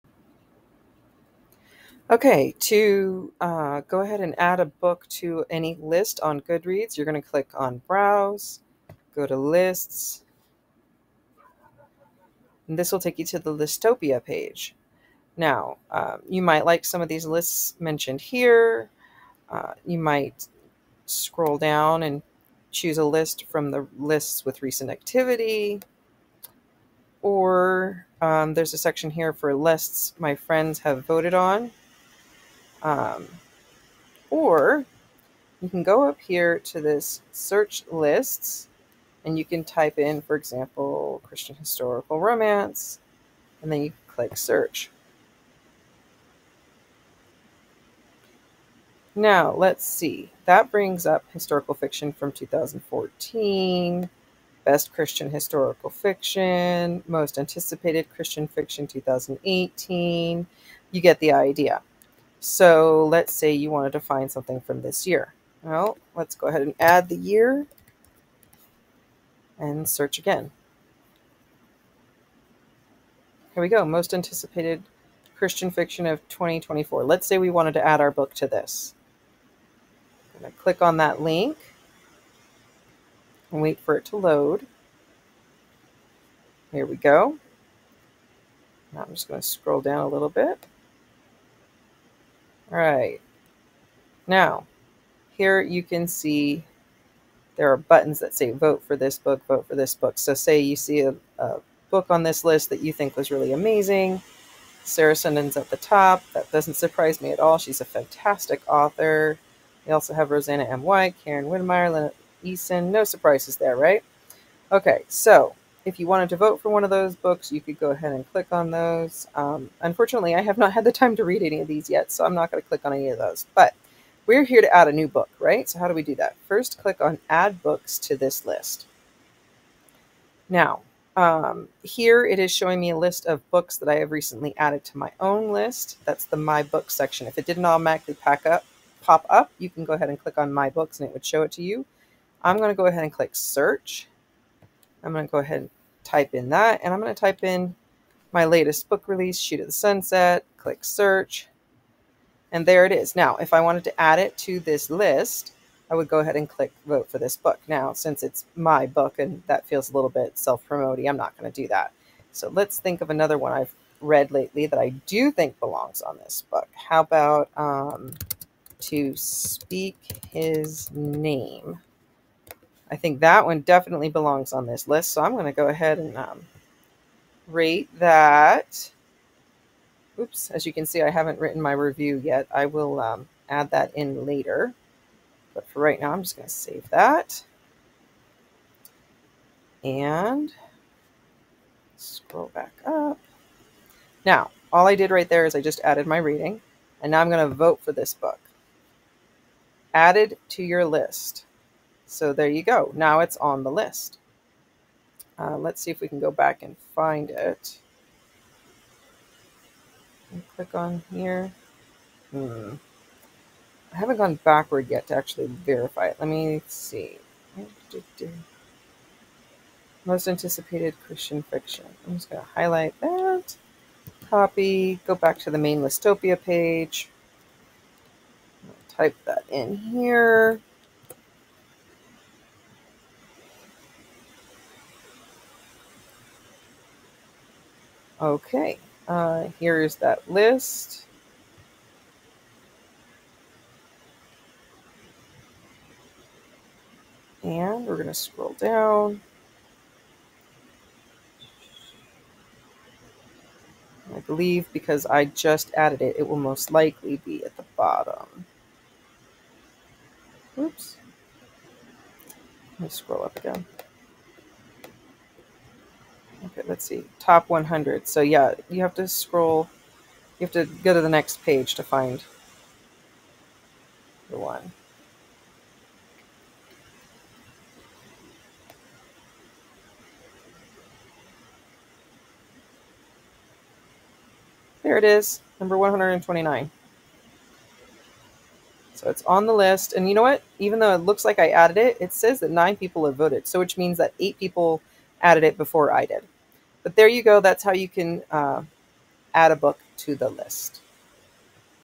How to add a book to a Goodreads Listopia List
This tutorial assumes that you already have a Goodreads account.